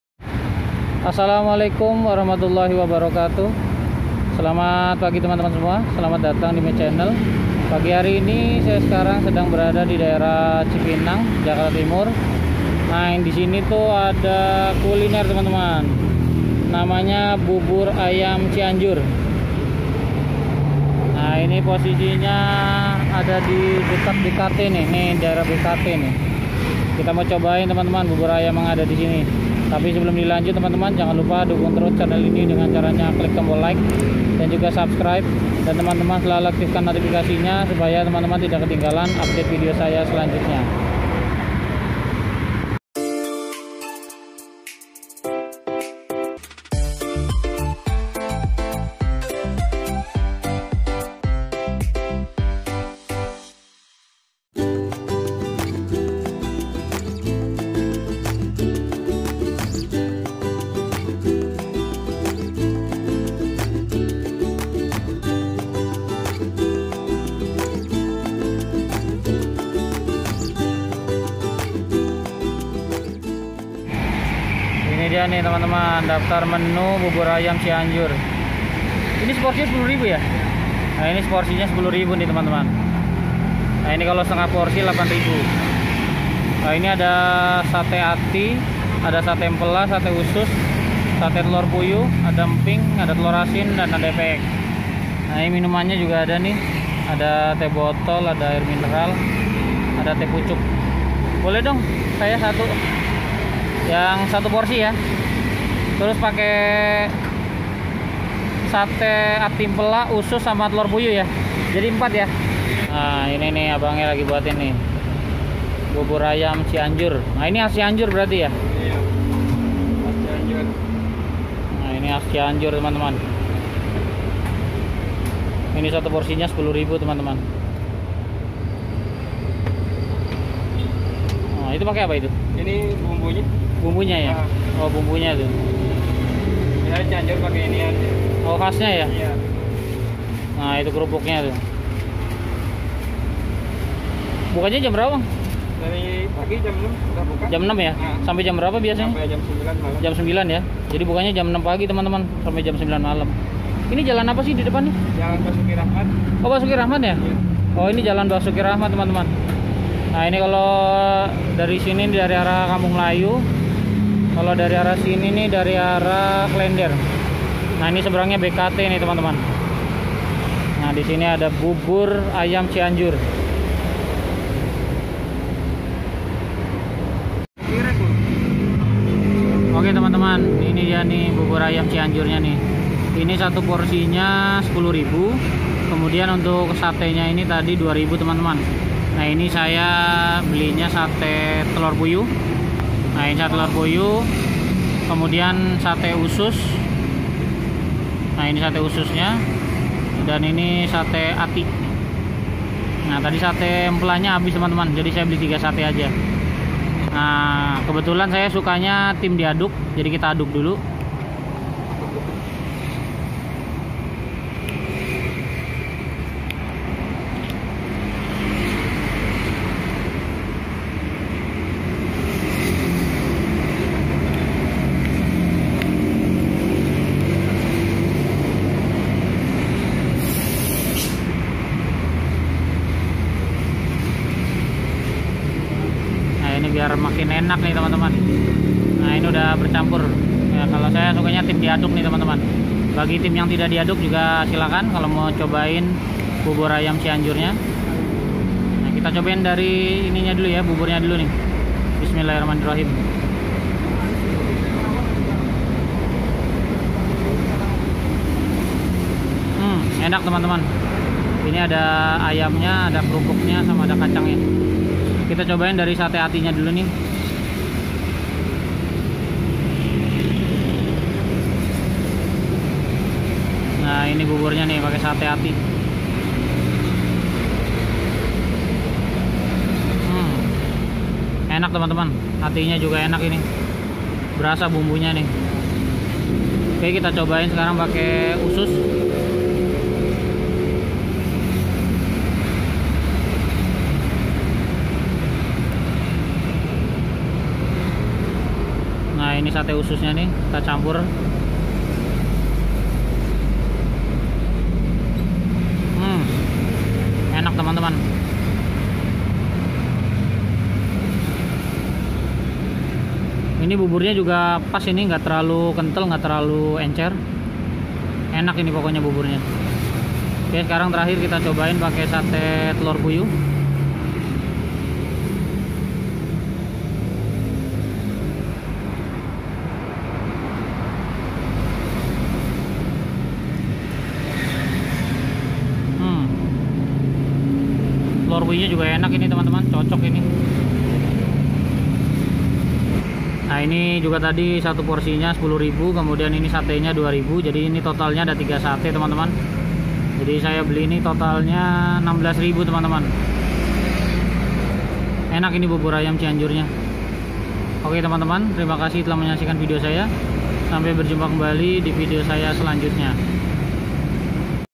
Bubur Ayam Cianjur | Indonesian Street Food
Bubur ayam cianjur ada disekitar daerah Cipinang, Jakarta Timur. Posisinya ada di Jalan Basuki Rahmat seberang BKT Cipinang.